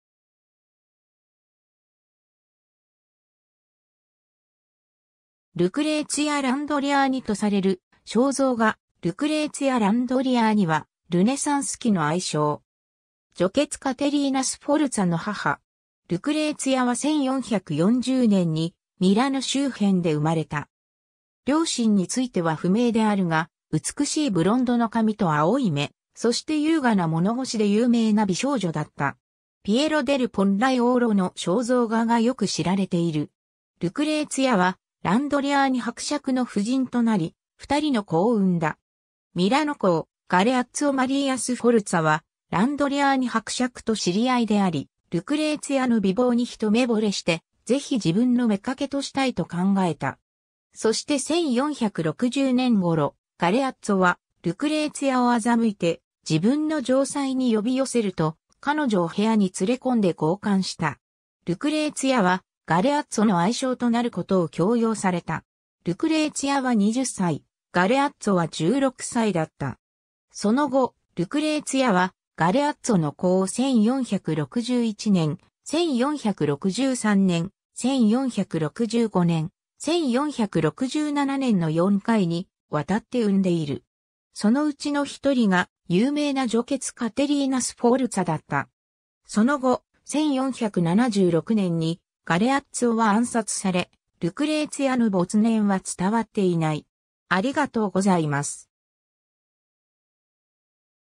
ルクレーツィア・ランドリアーニ
イタリア・ルネサンスの人物
ルクレーツィア・ランドリアーニとされる肖像画（ピエロ・デル・ポッライオーロ画、絵画館 (ベルリン)） ルクレーツィア・ランドリアーニ（Lucrezia Landriani 1440年 - 没年不詳）は、ルネサンス期の愛妾。
女傑カテリーナ・スフォルツァの母。
 ルクレーツィアは1440年にミラノ周辺で生まれた。
両親については不明であるが、美しいブロンドの髪と青い目、そして優雅な物腰で有名な美少女だった。
ピエロ・デル・ポッライオーロの肖像画がよく知られている。
 ルクレーツィアはランドリアーニ伯爵の夫人となり、二人の子を生んだ。
ミラノ公ガレアッツォ・マリーア・スフォルツァはランドリアーニ伯爵と知り合いであり、ルクレーツィアの美貌に一目惚れしてぜひ自分の妾としたいと考えた。
そして1460年頃、ガレアッツォはルクレーツィアを欺いて自分の城砦に呼び寄せると、彼女を部屋に連れ込んで強姦した。
ルクレーツィアはガレアッツォの愛妾となることを強要された。
ルクレーツィアは20歳、ガレアッツォは16歳だった。
その後、ルクレーツィアはガレアッツォの子を1461年、1463年、1465年、1467年の四回にわたって生んでいる。
そのうちの一人が、有名な女傑カテリーナ・スフォルツァだった。
 その後、1476年にガレアッツオは暗殺され、ルクレーツィアの没年は伝わっていない。